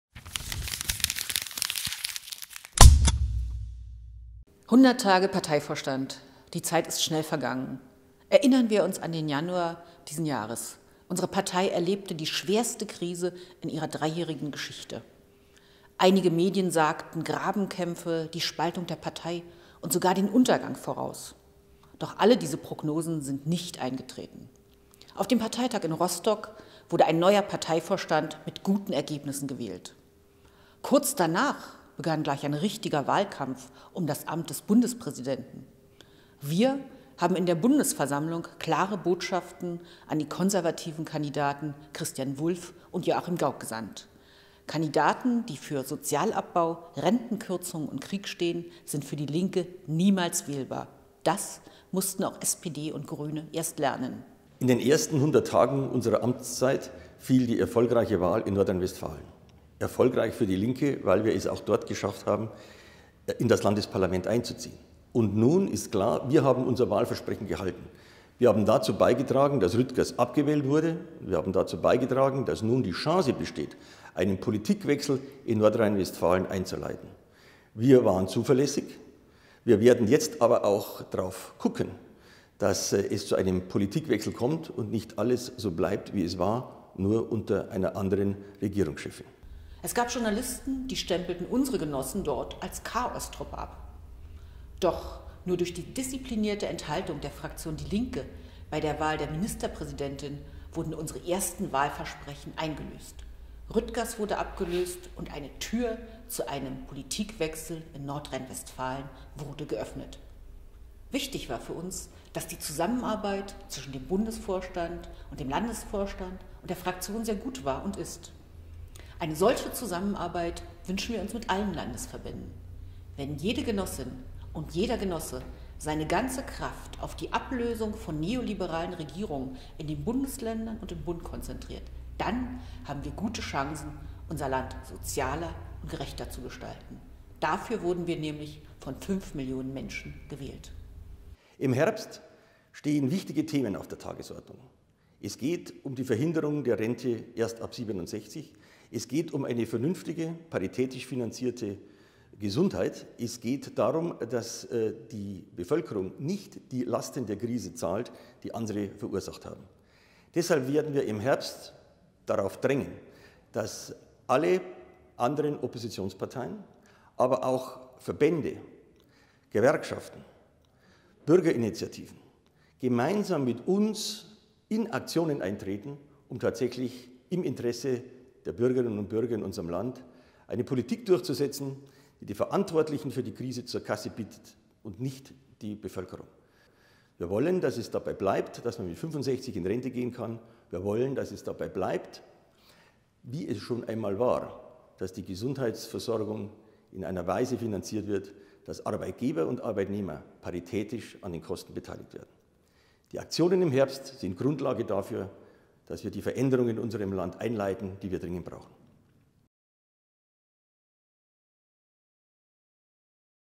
100 Tage: Eine Bilanz von Klaus Ernst und Gesine Lötzsch
Vor etwas mehr als 100 Tagen wählte der Rostocker Parteitag der LINKEN Klaus Ernst und Gesine Lötzsch zu den Parteivorsitzenden der LINKEN. Die sonst übliche Schonzeit von 100 Tagen konnte sich die neue Doppelspitze der LINKEN nicht gönnen: Die überraschende Bundespräsidentenwahl, der Machtpoker in NRW und vor allem der Kampf gegen die unsoziale Politik der Bundesregierung unter anderem mit Rente ab 67 und Sparpaket forderte DIE LINKE. Für den Herbst kündigen Lötzsch und Ernst gemeinsam mit Gewerkschaften, Sozialverbänden und Bewegung scharfe Proteste gegen die Sparpolitik an.